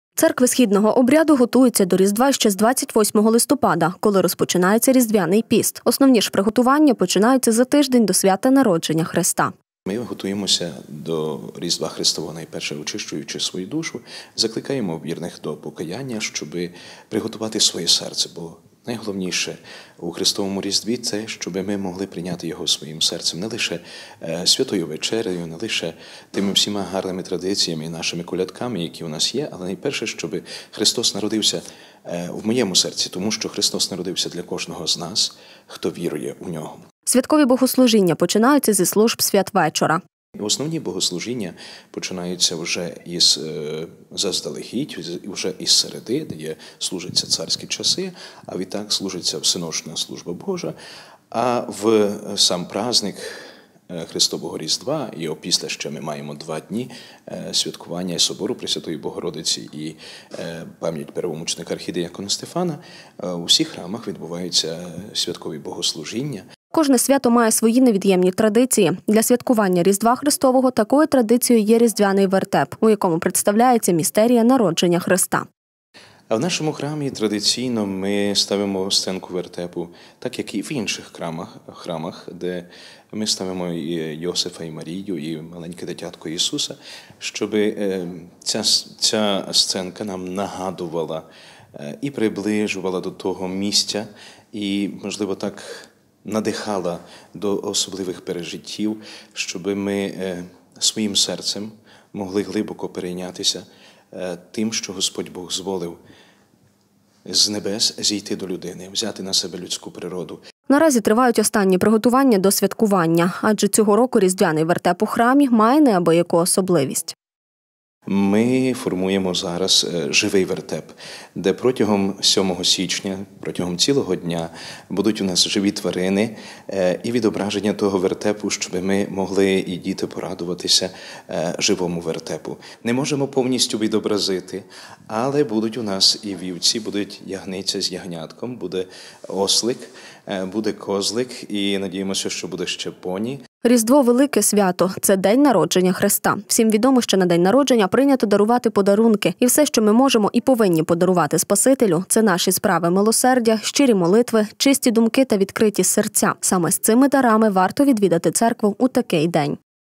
Оригінальний вертеп. На Закарпатті готуються до Різдва
Церкви східного обряду готуються до Різдва ще 28 листопада, коли розпочинається різдвяний піст. Основні ж приготування починаються за тиждень до свята народження Христа.Святкові богослужіння починаються зі служб Святвечора.Кожне свято має свої невід'ємні традиції. Для святкування Різдва Христового такою традицією є Різдвяний вертеп,  у якому представляєтья містерія Різдва Христового. Наразі у храмі тривають останні приготування до святкування. Адже цього року різдвяний вертеп у храмі має неабияку особливість.Різдво – велике свято, це день народження Христа. Всім відомо, що на день народження прийнято дарувати подарунки. І все , що ми можемо і повинні подарувати спасителю – це наші справи милосердя, щирі молитви, чисті думки та відкриті серця. Саме з цими дарами варто відвідати церкву у такий день.